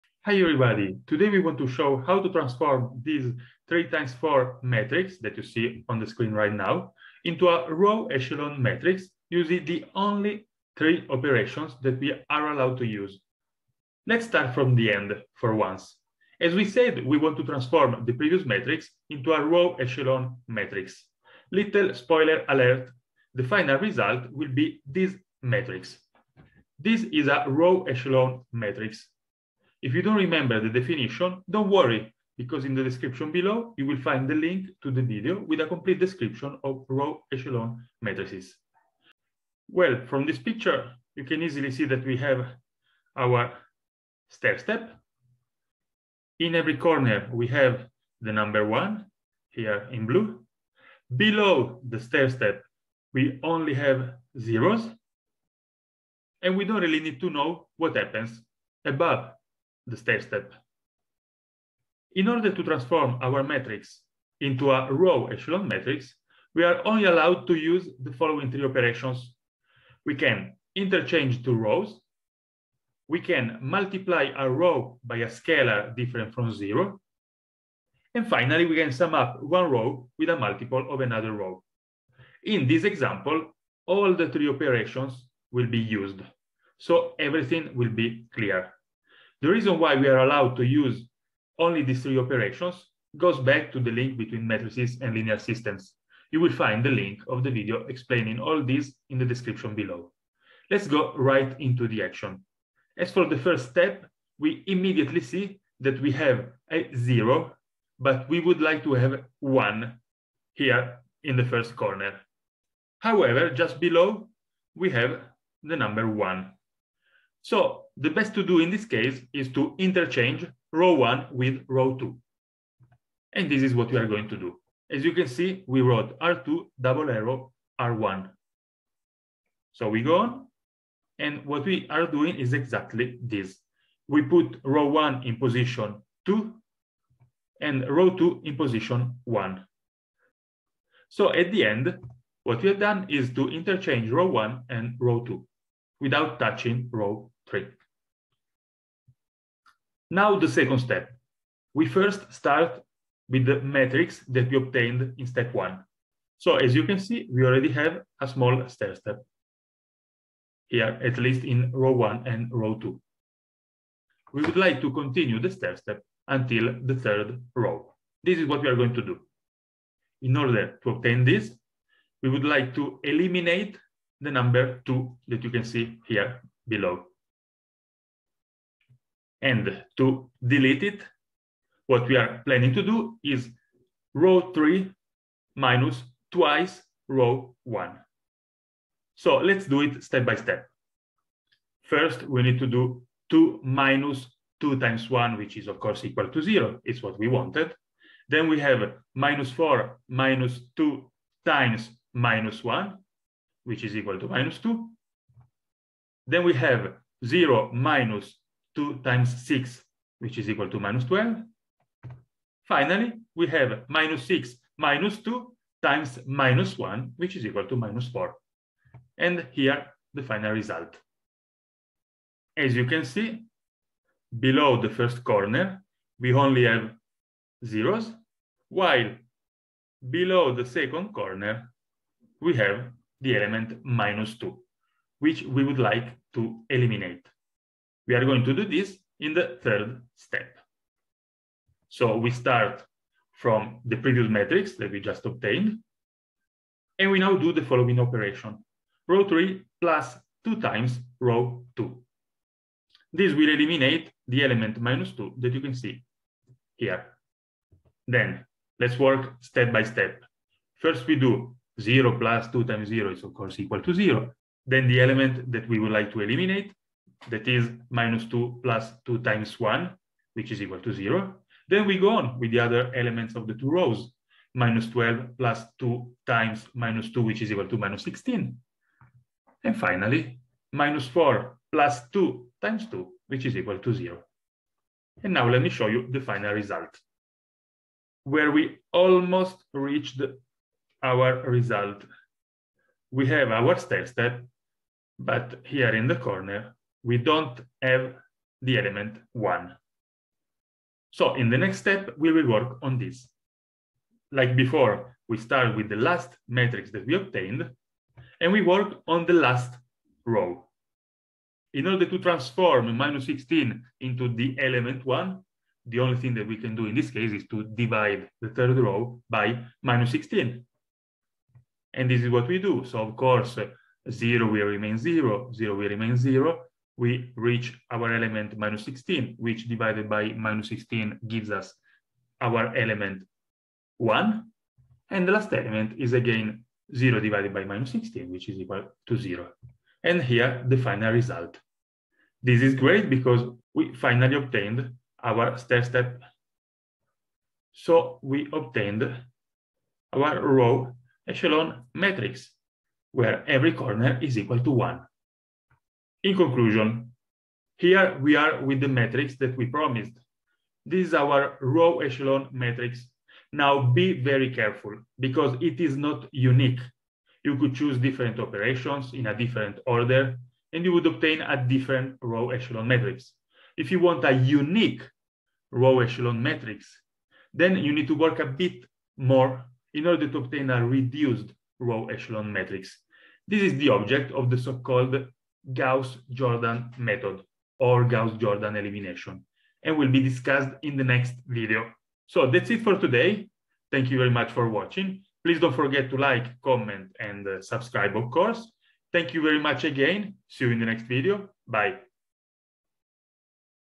Gaussian elimination (How to transform a matrix to its row echelon form)
In this video we are providing an example of how to transform a 3x4 matrix into its "row echelon" form. This is what we call Gauss-Jordan elimination.

Here some videos that you may need to watch in order to better understand this one: